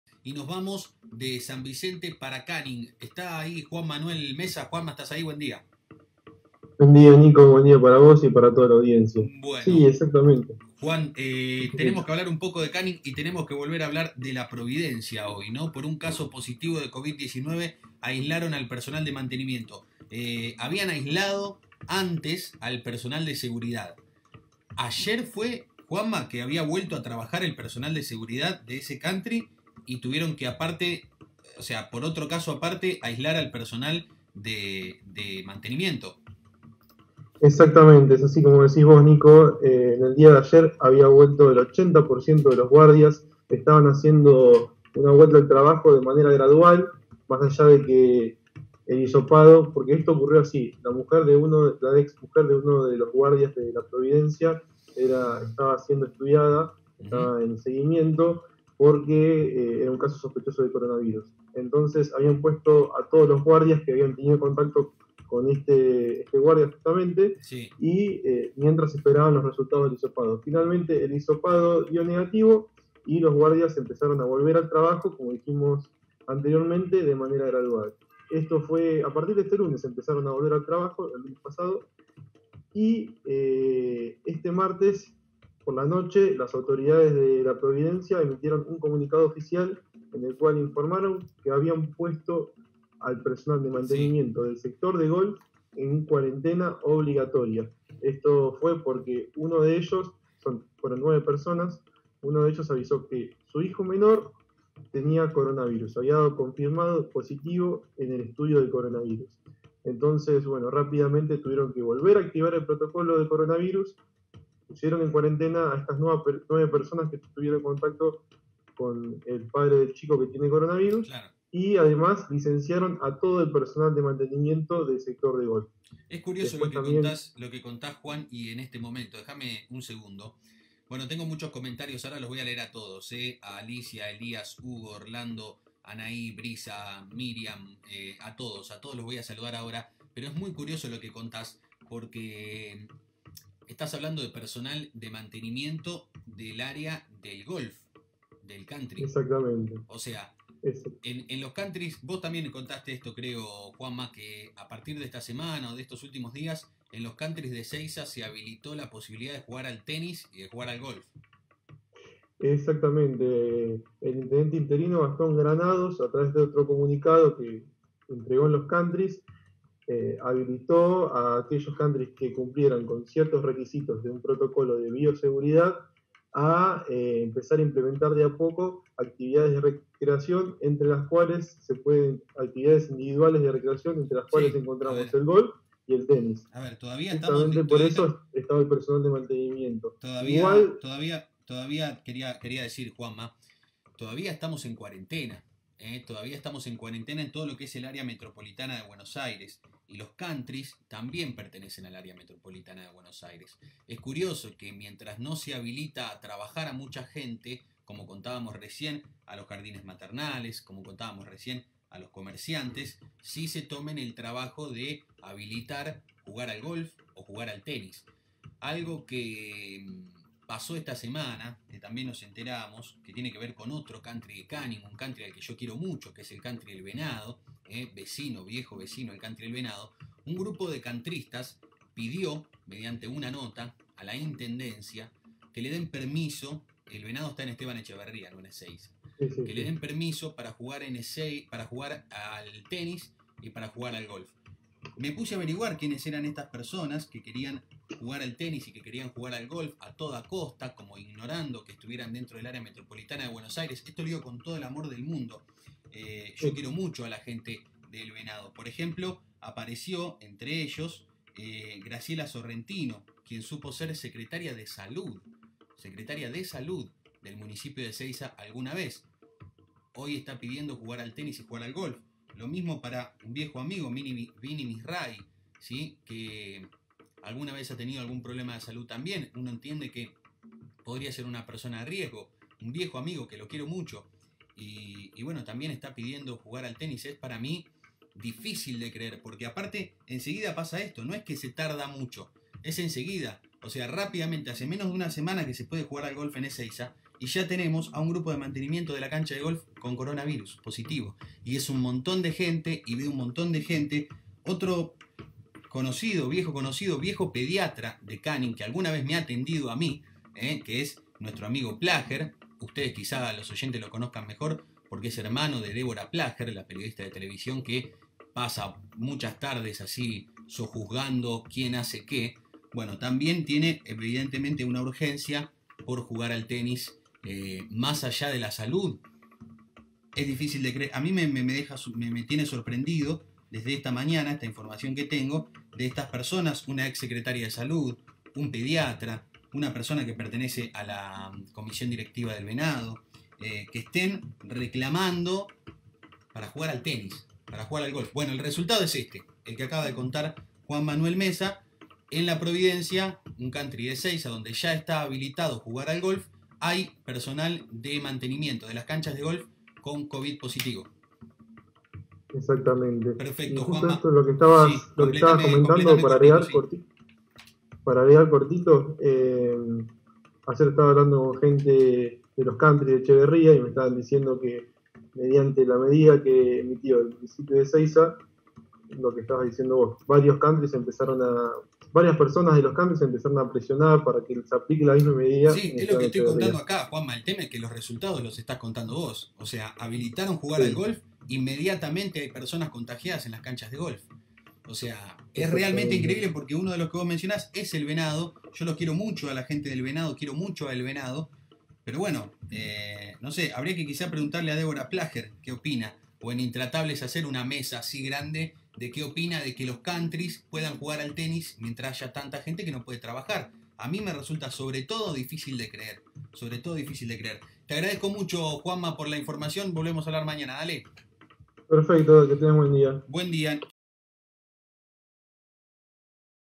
ElDiarioSur Canning: Polémica por un nuevo caso de Covid19 y la petición de habilitar deportes
ElDiarioSur en vivo DesdeLaRedacción. A pesar de la confirmación de un nuevo caso en La Providencia, varios countristas pidieron habilitar algunos deportes